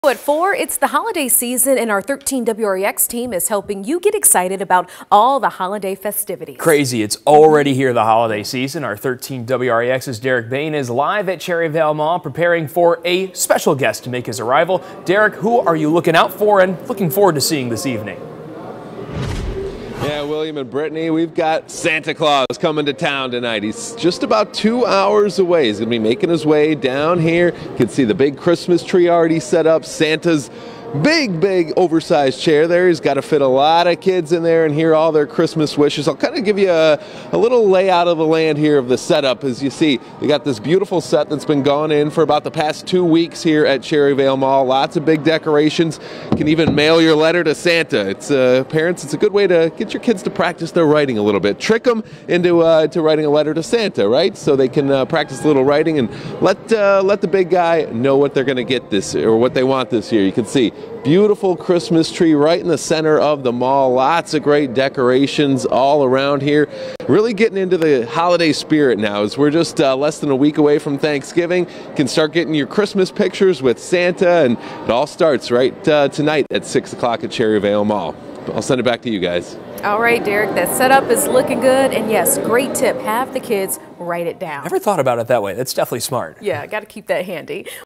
CherryVale Mall prepares for Santa's arrival
13 WREX's Derek Bayne reports live from CherryVale Mall ahead of Santa's arrival.